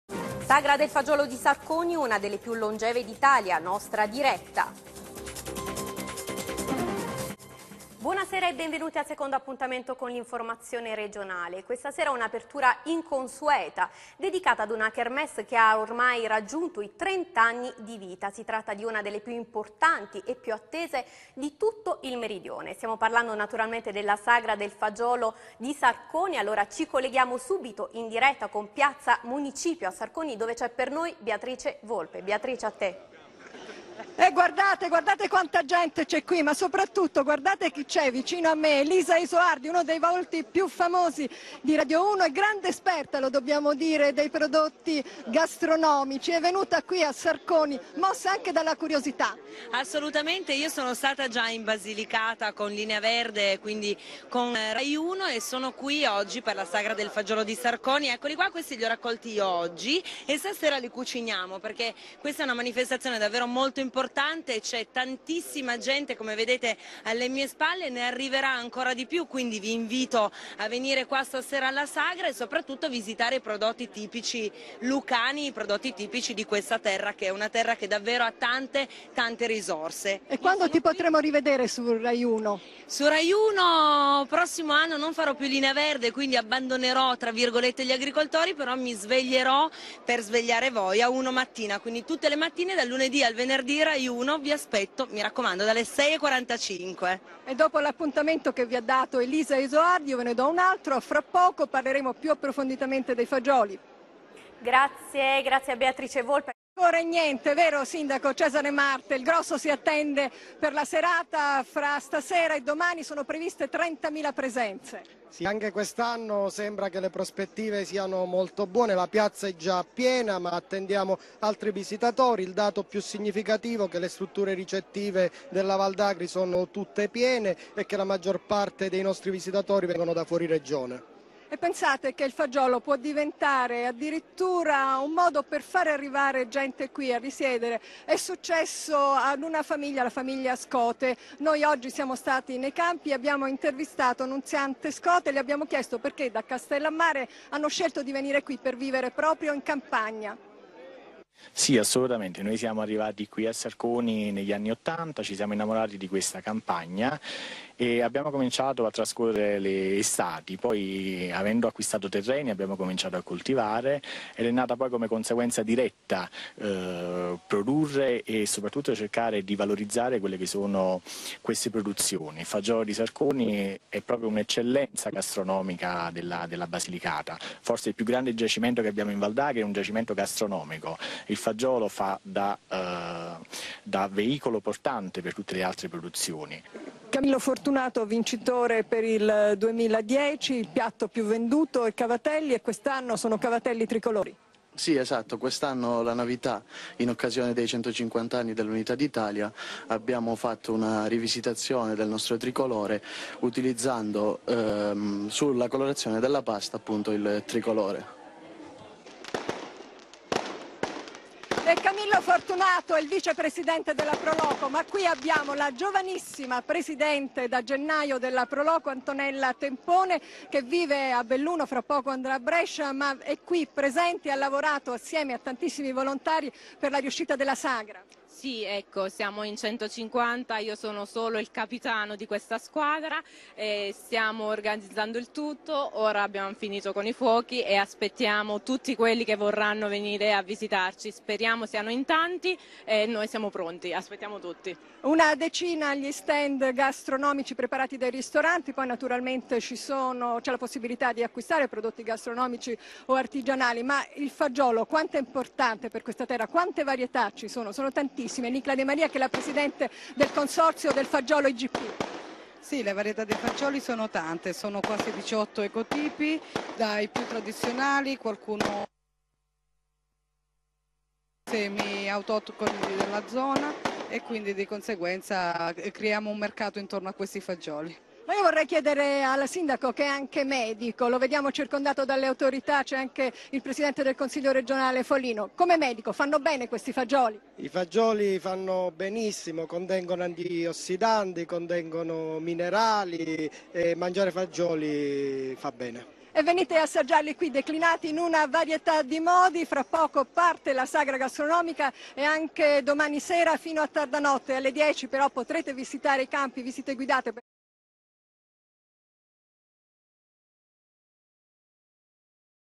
Diretta Rai XXX Sagra Del Fagiolo Igp di Sarconi
SARCONI 18-19 AGOSTO

Trenta ma non li dimostra. Tanti sono gli anni che Sarconi ospita la Sagra del Fagiolo. Un paese dai vicoli meravigliosi, pieno di storia e tradizioni, che in agosto per due giorni diventa il regno della buona tavola e del folklore in onore del suo prezioso legume.
Tutto per la strada, rigorosamente senza biglietto d'ingresso. Tutto "ispirato" al fagiolo, e al fascino semplice e genuino di un prodotto e di una comunità effervescenti. Tutti "inebriati" dai sapori di Sua Maestà il Fagiolo, che in giro per il centro storico si concede ai suoi sudditi più fedeli fino a tarda notte. Ecco la nostra sagra, un grande menù dall'antipasto al dolce tutto a base di fagioli.

Provate a farvi coinvolgere, provate a lasciarvi andare. Capirete che raggiungerci forse non è stato facile: ma partire sarà molto più difficile.

Ormani ci siamo... è tutto pronto
Vi aspettiamo numerosi a SARCONI "Capitale Europea del Fagiolo"